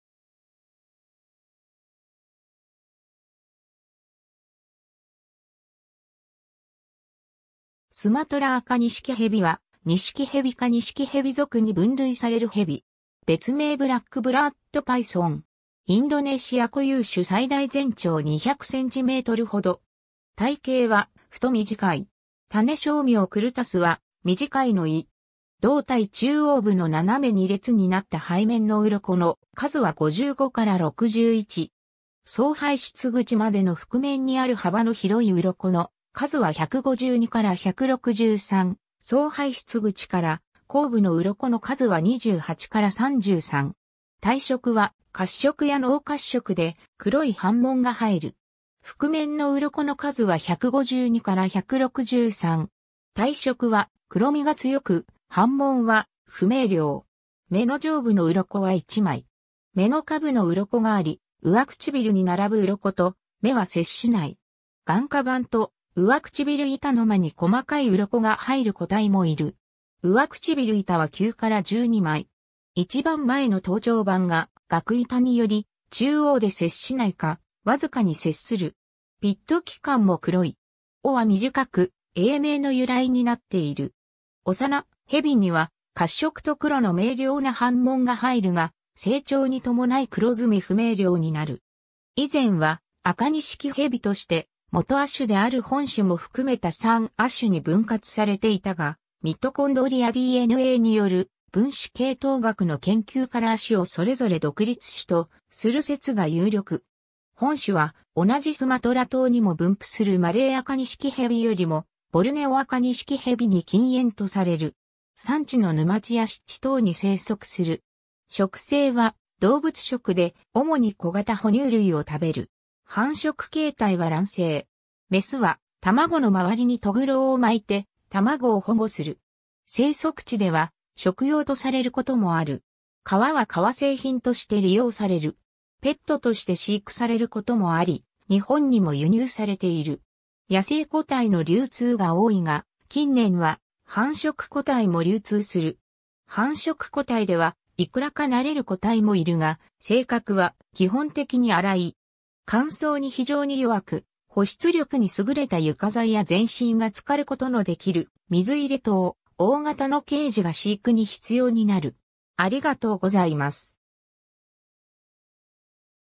スマトラアカニシキヘビ
スマトラアカニシキヘビ（蘇門答刺赤錦蛇、学名：Python curtus）は、ニシキヘビ科（ボア科ニシキヘビ亜科とする説もあり）ニシキヘビ属に分類されるヘビ。
別名ブラックブラッドパイソン。
 インドネシア（スマトラ島西部および周辺の島嶼）固有種 最大全長200cm程。
体形は太短い。
種小名curtusは「短い」の意。
胴体中央部の斜めに列になった背面の鱗の数（体列鱗数）は55-61。
総排出口までの腹面にある幅の広い鱗の数（腹板数）は152-163、総排出口から後部の鱗の数（尾下板数）は28-33。
体色は褐色や濃褐色で、黒い斑紋が入る。
腹面の鱗の数（腹板）は152-163。
体色は黒味が強く、斑紋は不明瞭。
 眼の上部の鱗（眼上板）は1枚。
眼の下部の鱗（眼下板）があり、上唇に並ぶ鱗（上唇板）と眼は接しない。
眼下板と上唇板の間に細かい鱗が入る個体もいる。
上唇板は9-12枚。
一番前の頭頂板が額板により中央で接しないか、僅かに接する。
ピット器官も黒い。
尾は短く、英名（short-tailed）の由来になっている。
 幼蛇には褐色と黒の明瞭な斑紋が入るが、成長に伴い黒ずみ不明瞭になる。
 以前はアカニシキヘビとして基亜種である本種も含めた3亜種に分割されていたが、ミトコンドリアDNAによる分子系統学の研究から亜種をそれぞれ独立種とする説が有力。
本種は同じスマトラ島にも分布するマレーアカニシキヘビよりもボルネオアカニシキヘビに近縁とされる。
 山地の沼地や湿地等に生息する。
 食性は動物食で、主に小型哺乳類を食べる。
メスは卵の周りにとぐろを巻いて、卵を保護する。
 生息地では食用とされることもある。
皮は革製品として利用される。
野生個体の流通が多いが、近年は繁殖個体も流通する。
繁殖個体ではいくらか慣れる個体もいるが、性格は基本的に荒い。
（他のアカニシキヘビ類よりは大人しいともされる。）乾燥に非常に弱く、保湿力に優れた床材や全身が浸かる事のできる水入れ等、大型のケージが飼育に必要になる。